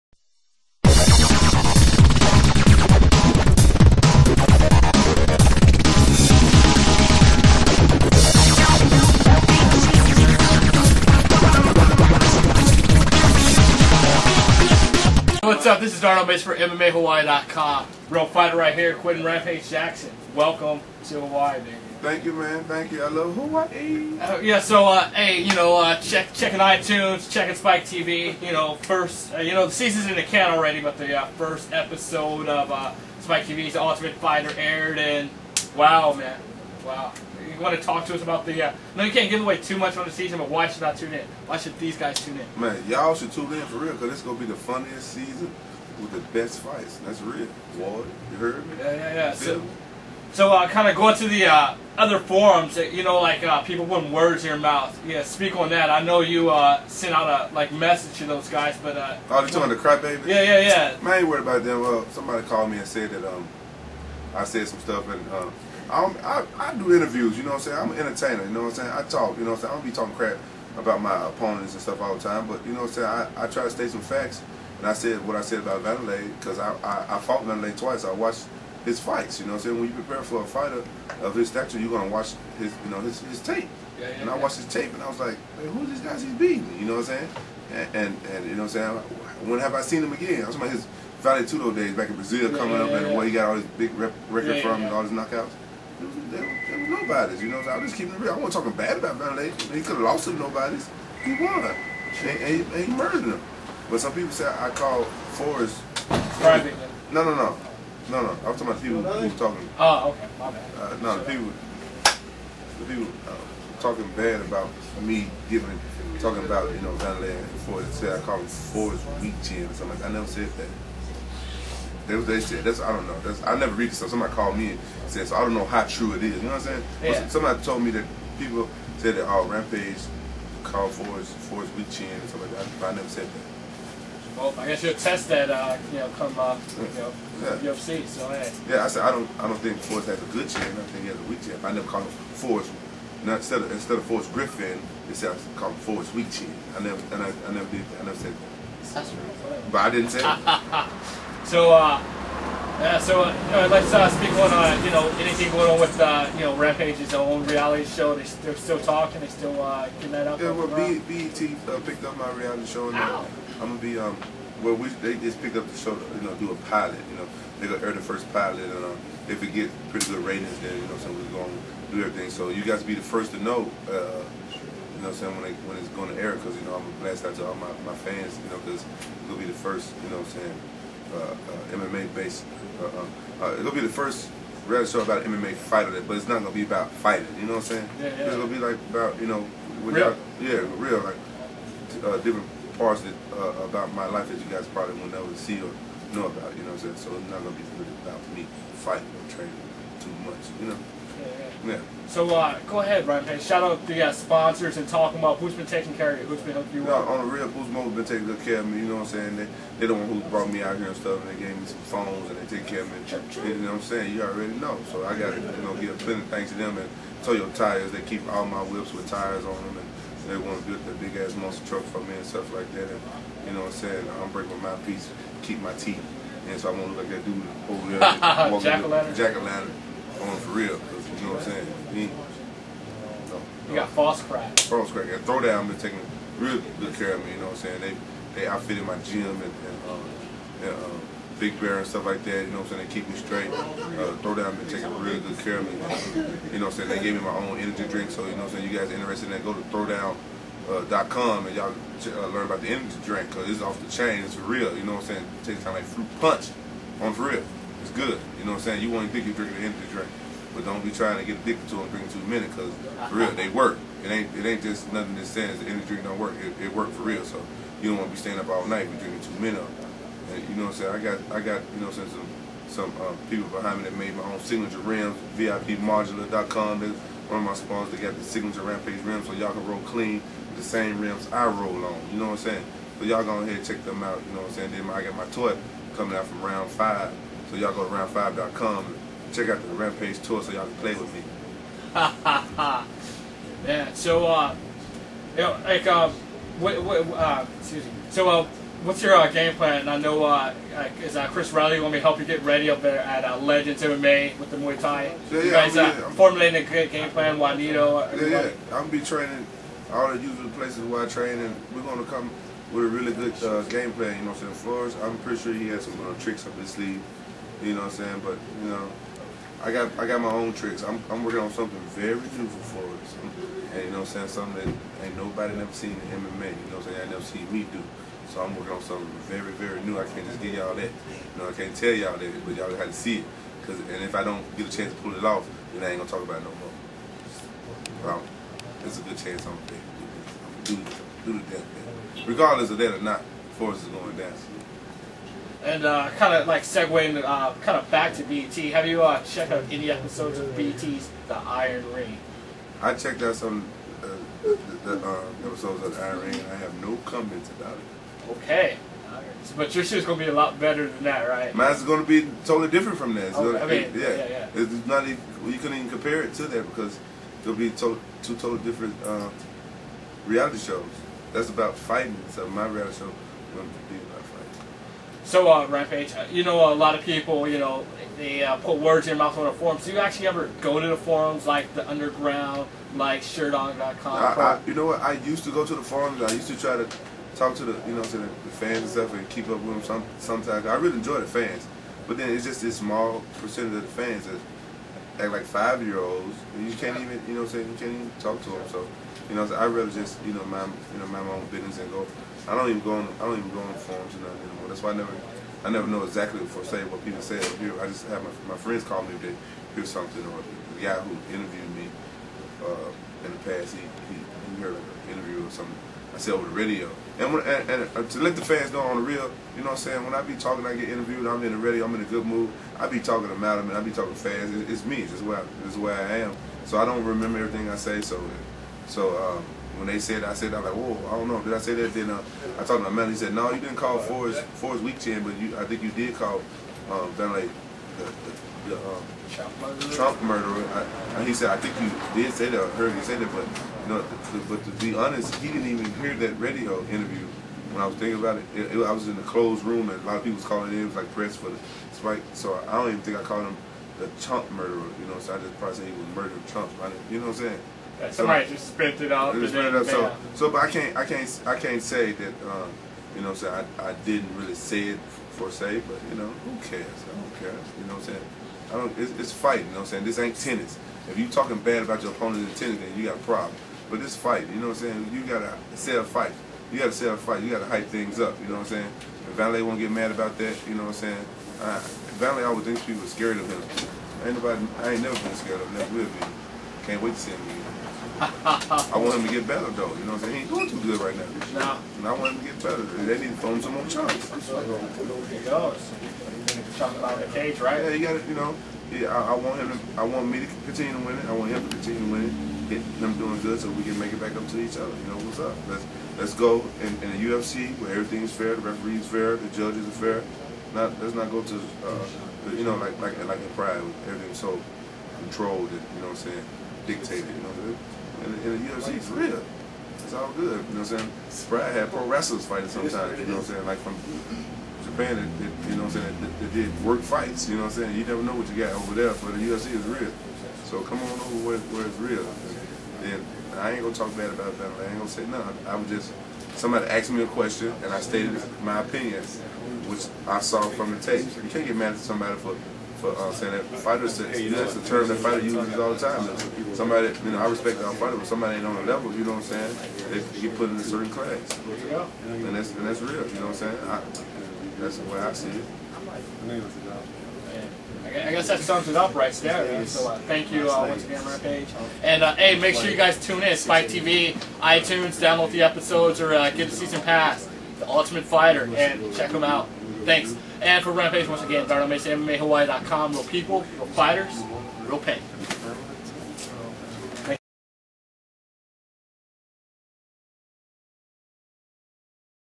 MMA Hawaii.com Interview With Quinton "Rampage" Jackson
Rampage drops in to speak on a variety of topics.

[Both Rampager and I are pretty jet-lagged in this clip, he having landed only a few hours before me. He's really cool, and I call this take one, as this was hastily shot before I got a chance to hang out. Again, really cool, able to out club me later that night (the next morning), andstill show up to cheer on the keiki (kids) at a grappling tournament, andI thought Tracy Lee didn't sleep. Stad by for take 2].